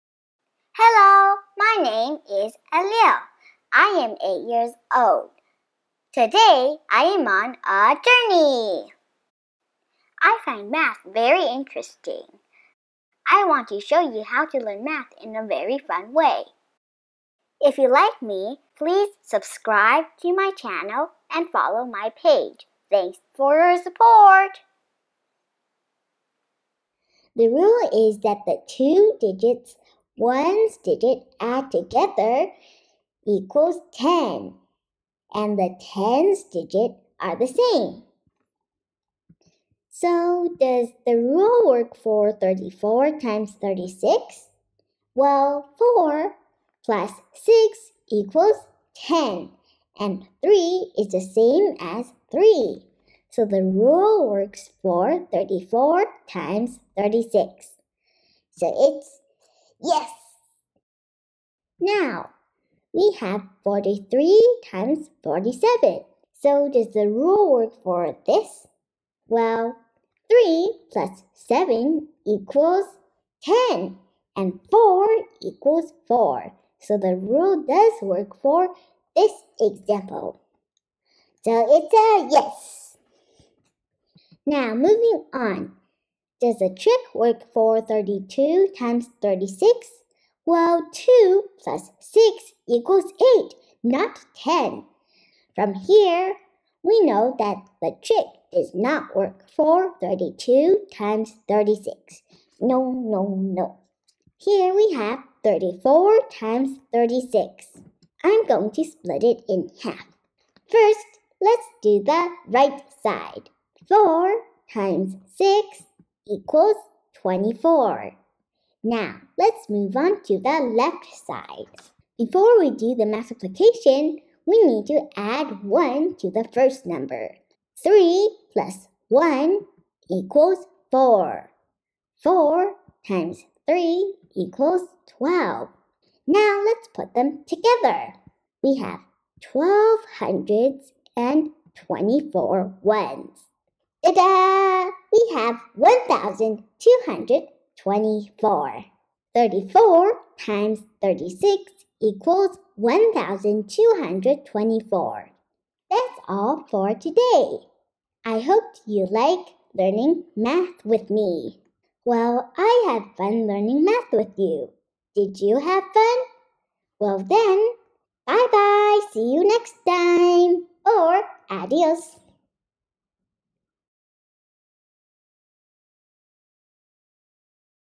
Fast Math Tricks | How to multiply 2 digit numbers in an easy way | Math Fun Lesson Introduction 47
Hello, my name is Alio, I find Math very interesting and would like to show you how I learn Math in a fun way. This short video shows you a trick so that you can quickly do the multiplication for the numbers following some rules.

What is 43 x 47? and what is 62 x 68? We can multiply some 2 digit numbers quickly for numbers meeting the following rules.
- The Sum of Ones Digits of the two numbers is 10
- The Number of the Tens Digits is the same